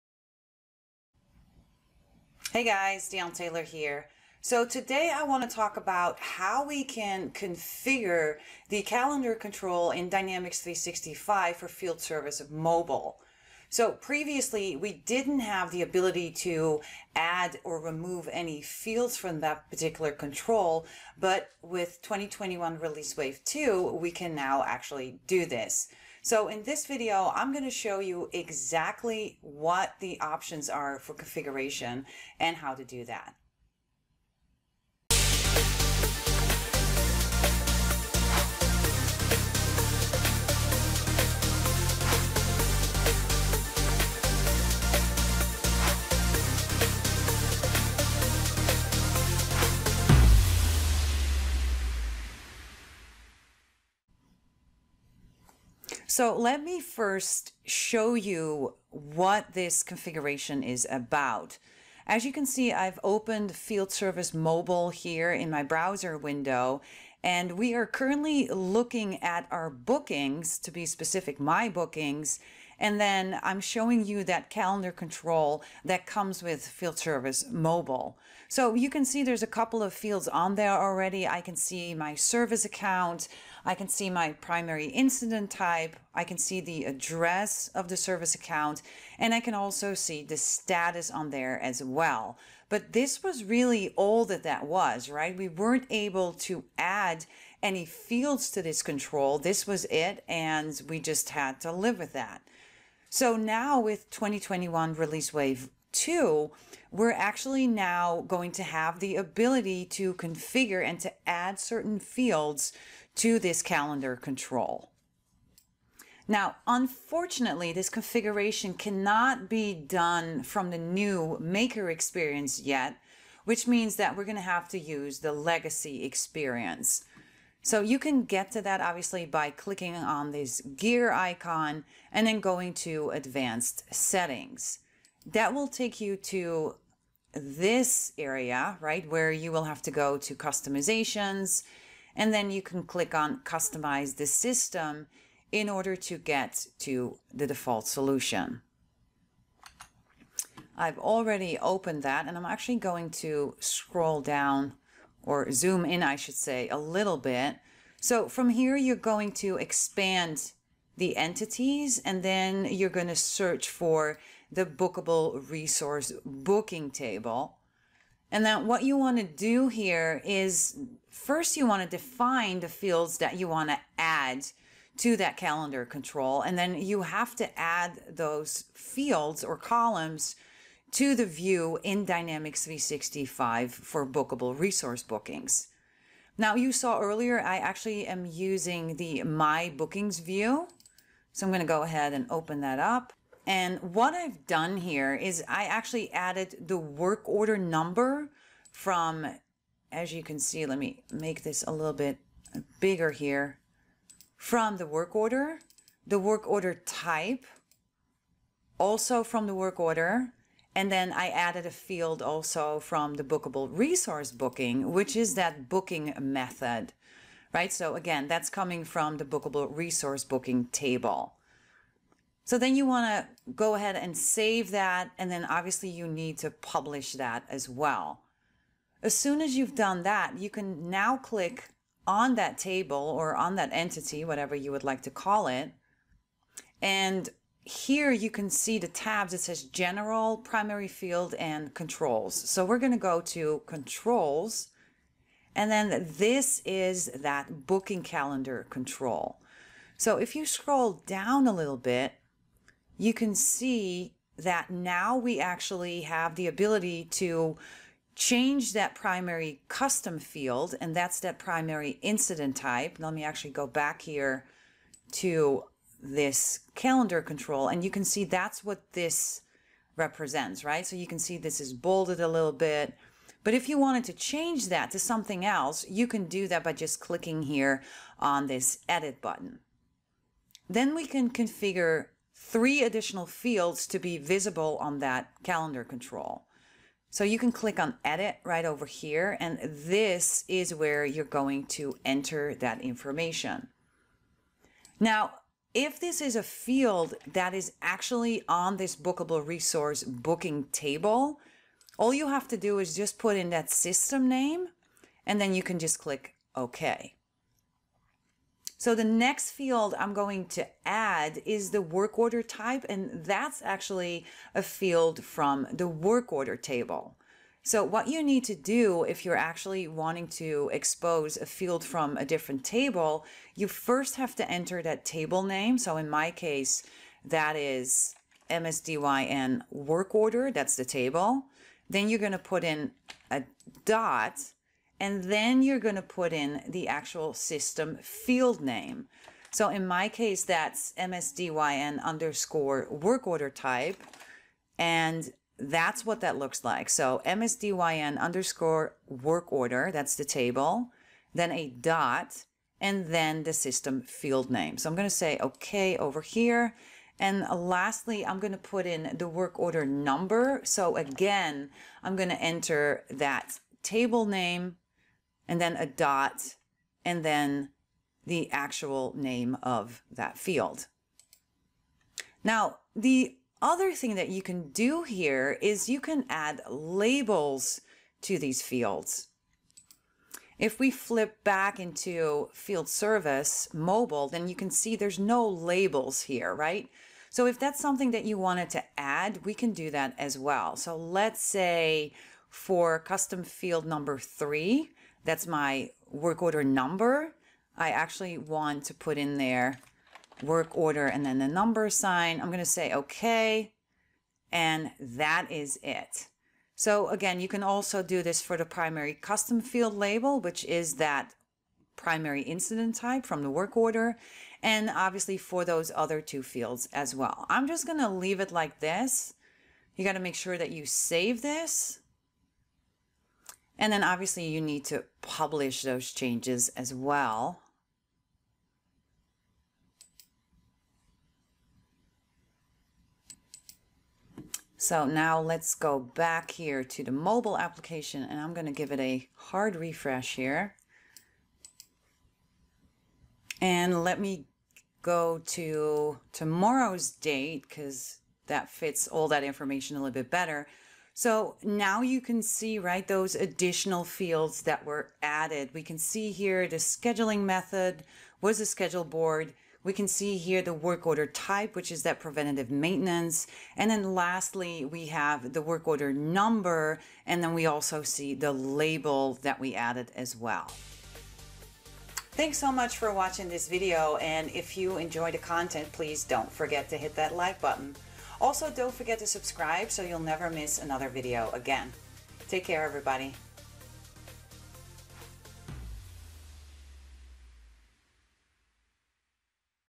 Customizing the booking calendar control in the Field Service mobile app
In this video I will show you how to configure the booking calendar control for the Dynamics 365 Field Service mobile app.